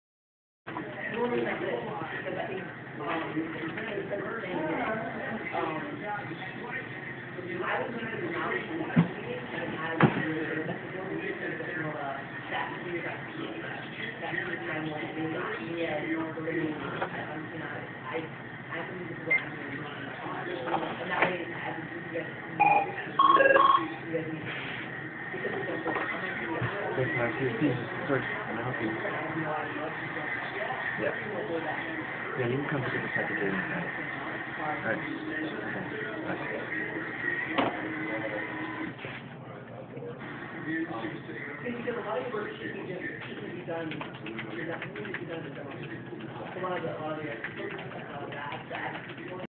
Edison Inspired Film - Office Work
Simulating Edison's early films, this video shows a young man doing daily office work. There isn't much spectacle and there is a minimum sense of depth. It was just filmed for the sake of motion pictures.